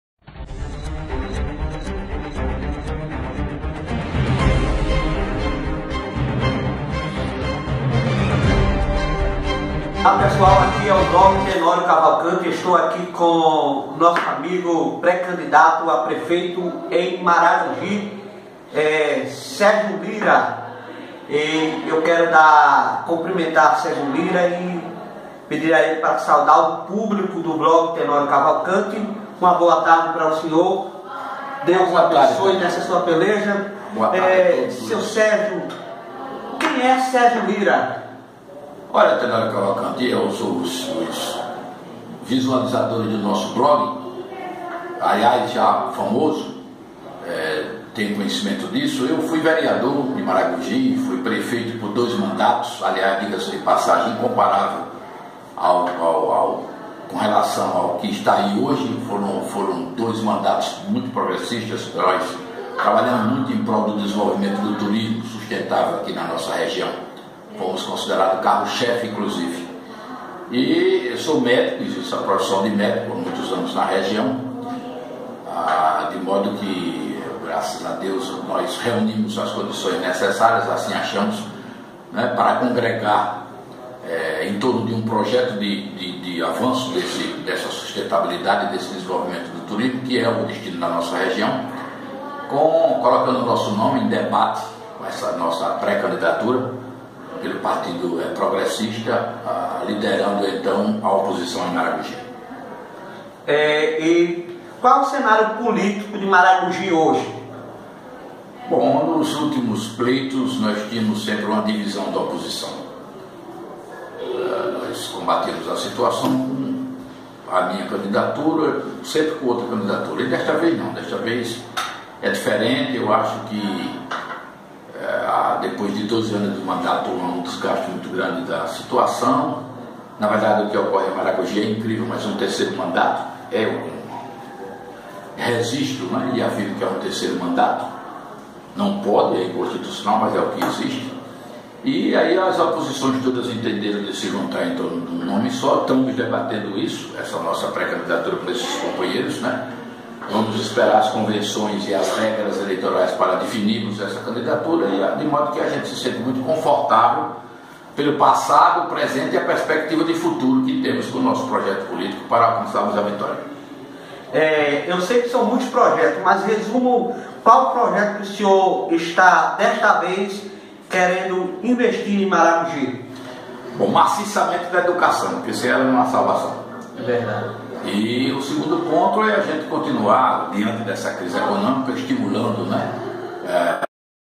Dr. Sergio Lira de Frente com o Blog Tenório Cavalcanti
Dr. Sergio Lira Pré Candidato de Maragogi de Frente com o Blog Tenório Cavalcanti, em uma papo de três minutos trás confiança administrativa para Maragogi-AL, em um papo tão rápido os eleitores já podem perceberem a segurança do que é liderança política.